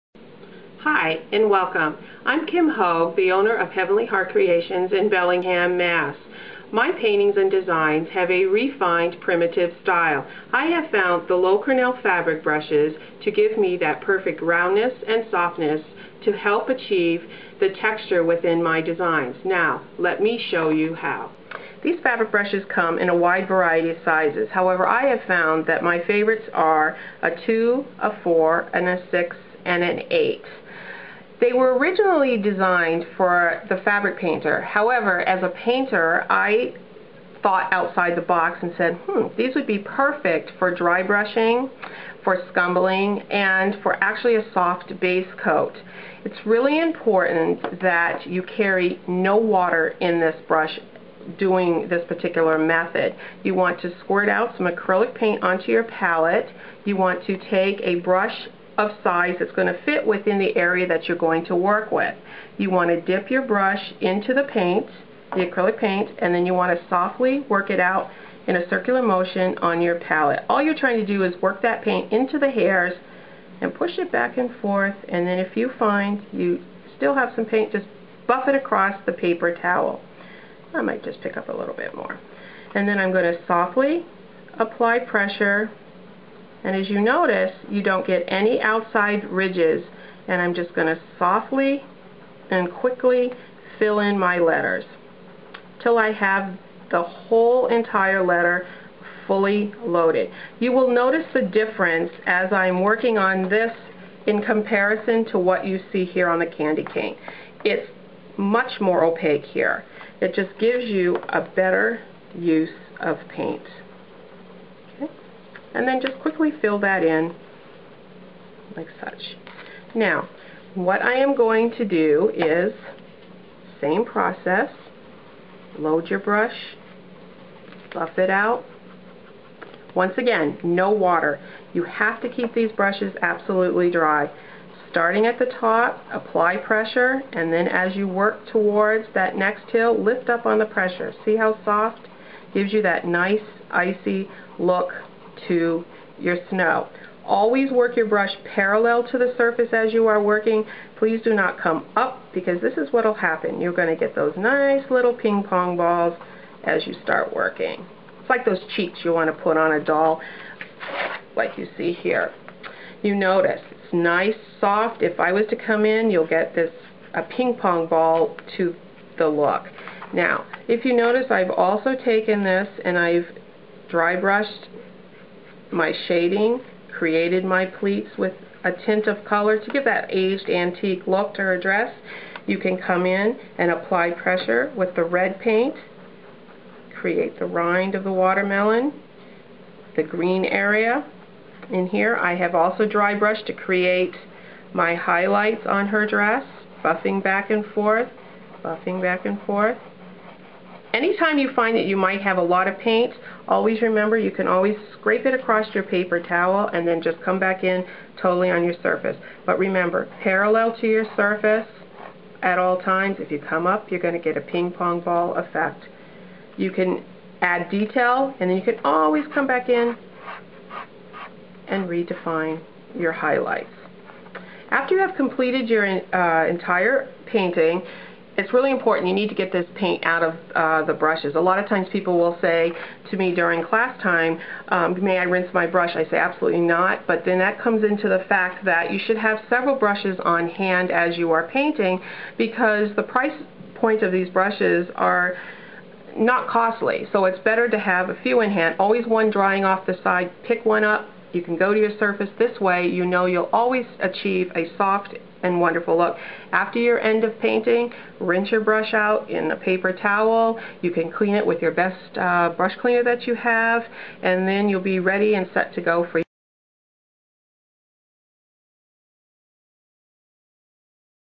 How-to Painting: Using Fabric Brushes for Drybrushing
Decorative Artist Kim Hogue shows how she uses to Loew-Cornell fabric brushes to drybrush and create highlights, shading and give texture to her paintings.